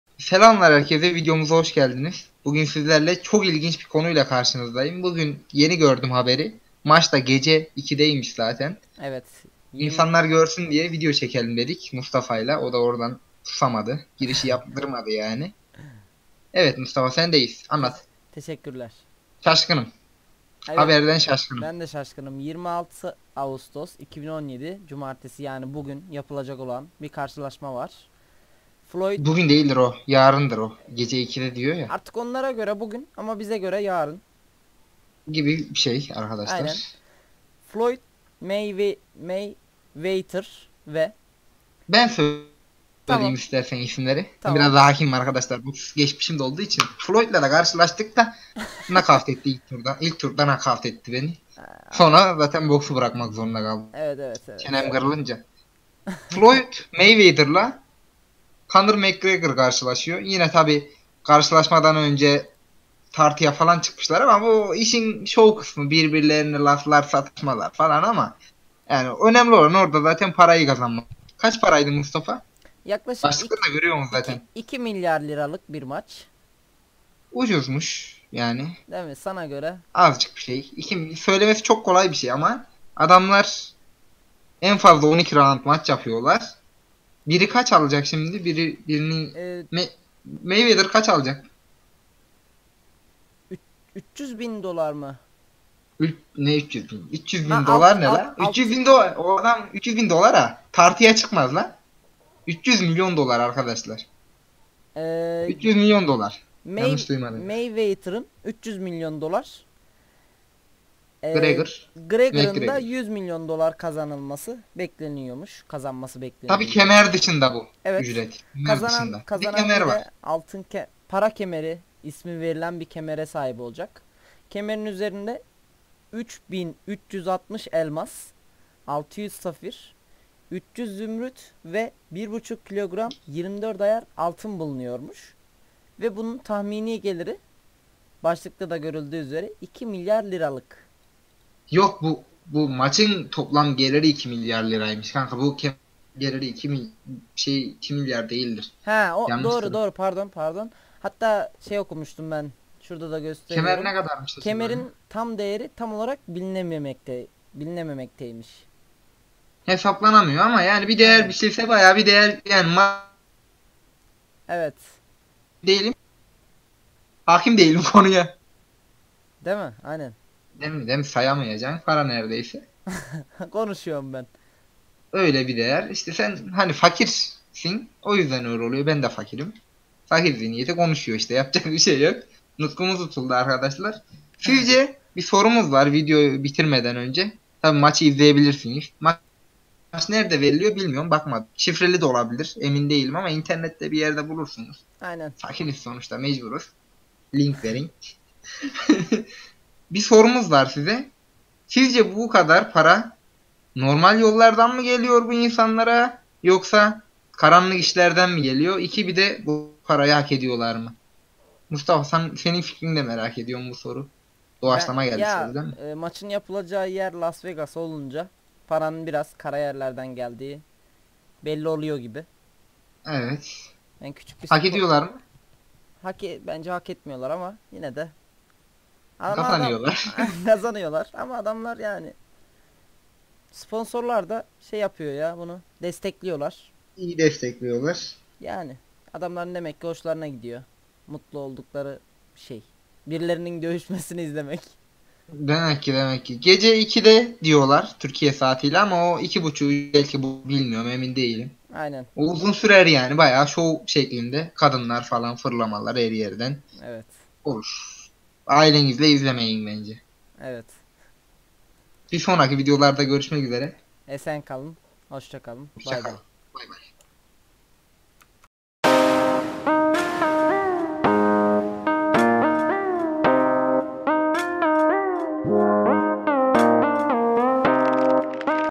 2 Milyar Tl'lik Boks Maçı (Floyd Mayweather - Conor McGregor )
bugün sizlerle boks tarihinin piyası değeri en yüksek maçlarından birine değindik iyi seyirler...

Bu arada maç türkiye saati ile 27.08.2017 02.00 da S Spor kanalından yayınlanacaktır

Dünya boks tarihinin önde gelen isimleri arasında yer alan Floyd Mayweather ve Conor McGregor karşı karşıya geliyor.Büyük merakla beklenen karşılaşma Las Vegas'ta gerçekleşecek. Sabaha karşı 02.00'de gerçekleşecek boks maçında dünya şampiyonluğu unvanı olmasa da Dünya Boks Konseyi'nin dev maça hazırladığı kemer dikkatleri üzerine çekiyor. 'Money Belt' (para kemeri) adı verilen kemerin üzerinde 3 bin 360 elmas, 600 safir, 300 zümrüt ve 1.5 kilogram 24 ayar altın bulunuyor.Mücadele öncesi tartıda yüz yüze gelen boksörler iddialı konuştu. McGregor, rakibini 4 raunt içinde nakavt edeceğini söyledi. Floyd Mayweather ise maçın 12 raunt sürmeyeceğini belirtirken, nakavtın geleceği raundu tahmin etmekten kaçındı.220'den fazla ülkede yayınlanacak maç yaşatacağı heyecan kadar elde edilecek gelirle de konuşulur hale geldi.
BBC'ye göre TV yayın hakları, reklamlar, sponsorluk hakları ve bilet satışlarıyla birlikte gecede 600 milyon Dolar (466 milyon Sterlin) gelir bekleniyor. Gecede Mayweather'ın yaklaşık 300 milyon Dolar, McGregor'un ise 100 milyon Dolar kazanması bekleniyor.
Dev organizasyon bu rakamlarla birlikte 2015 yılında 'yüzyılın maçı' olarak adlandırılan Mayweather - Pacquiao maçını geride bırakmış olacak.